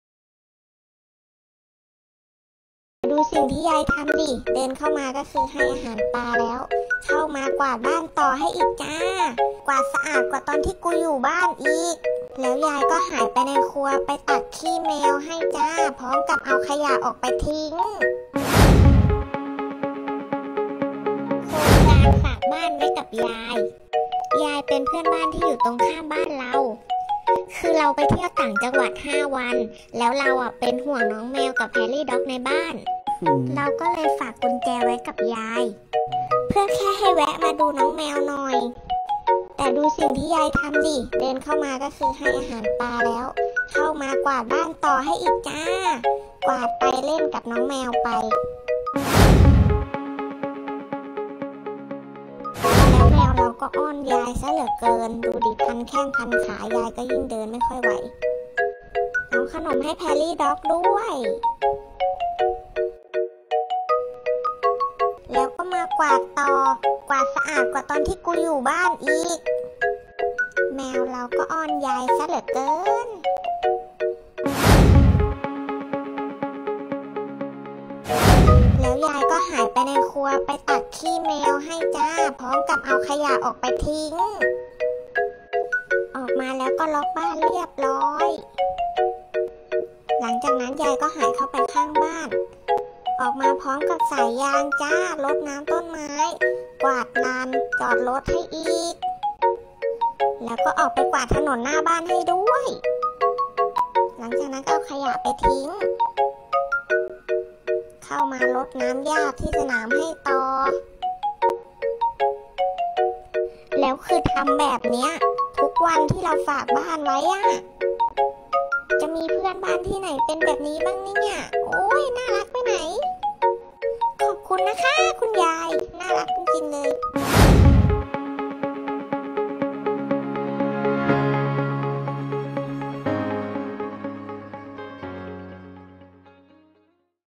โครงการฝากบ้านไว้กับเพื่อนบ้าน : [News story]
สาวแชร์เรื่องราวที่เหมือนถูกหวยรางวัลที่ 1 หลังเจอเพื่อนบ้านที่เป็นคุณยายสุดแสนน่ารัก โดยเจ้าของคลิปเล่าว่า ตนเองต้องไปต่างจังหวัดนานถึง 5 วัน จึงขอให้คุณยายมาช่วยให้อาหารสัตว์ที่เลี้ยงไว้ให้ แต่งานนี้คุณยายไม่ใช่แค่ให้อาหารอย่างเดียว แต่เป็นให้เกือบทุกอย่าง